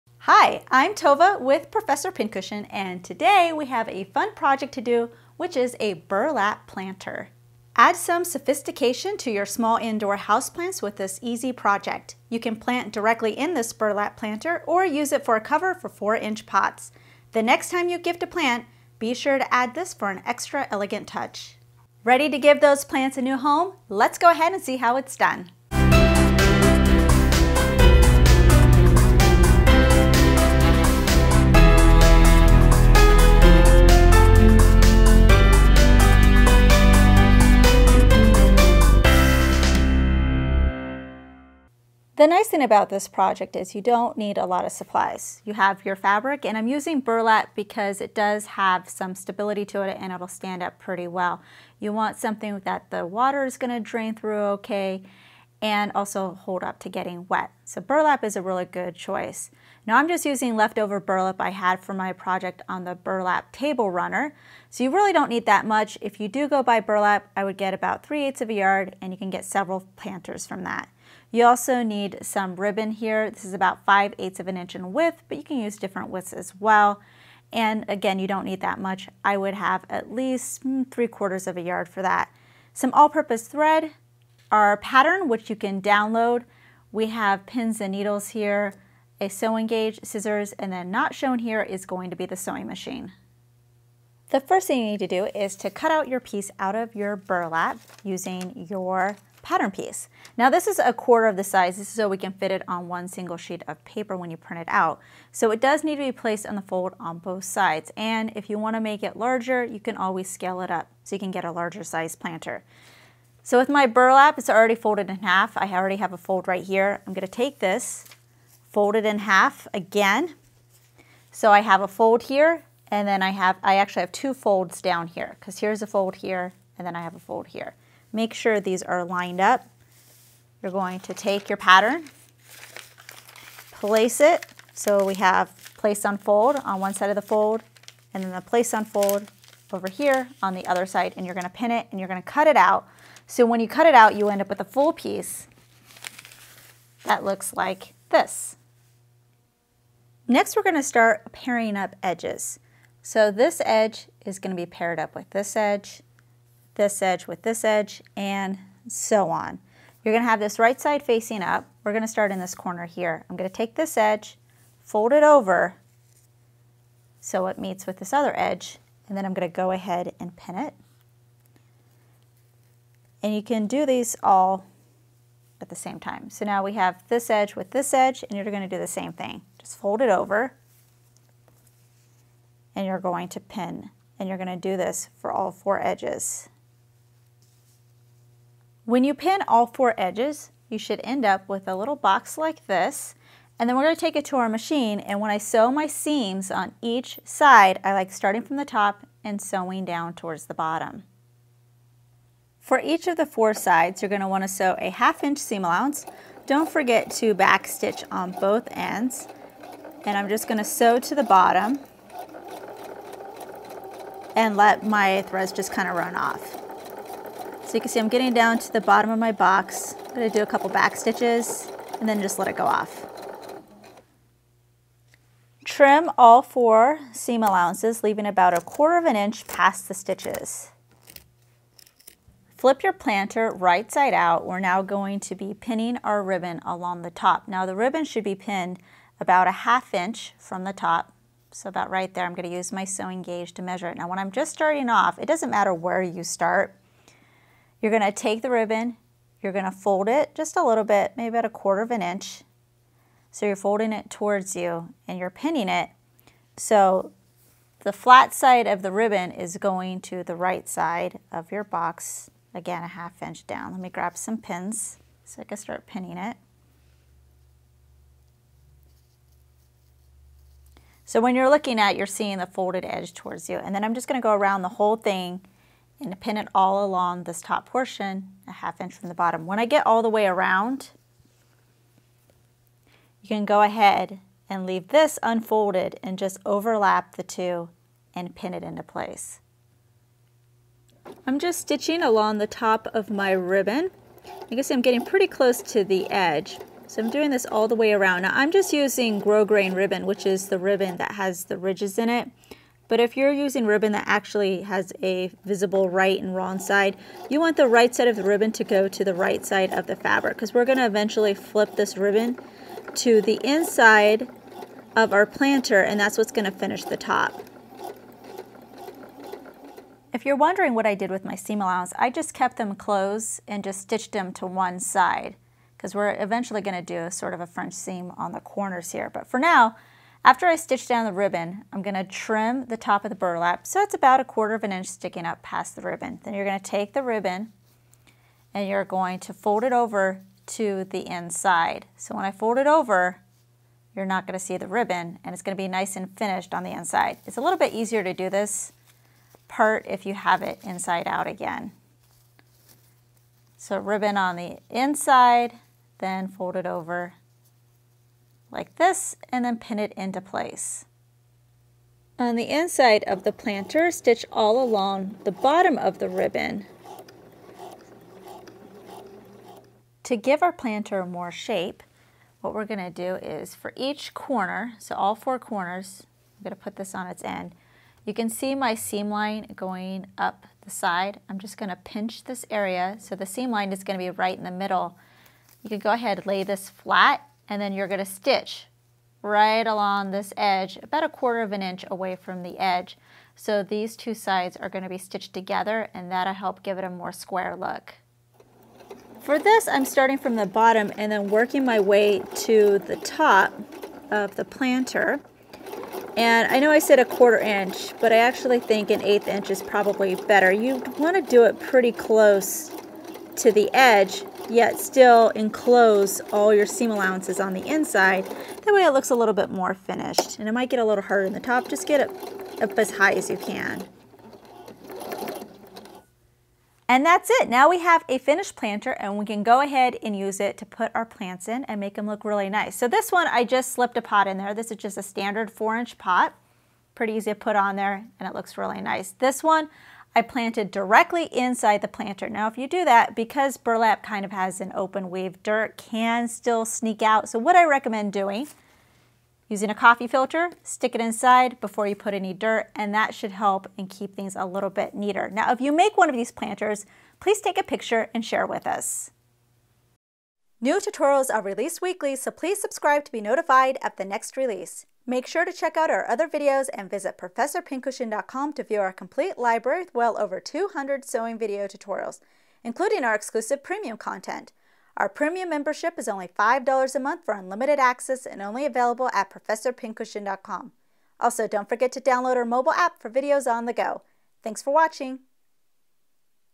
How to Sew a Burlap Planter
PATTERN:
(Please note you must be at least a FREE basic member on Professor Pincushion to download the pattern)

Give your small houseplants a new beautiful home in these elegant burlap planters.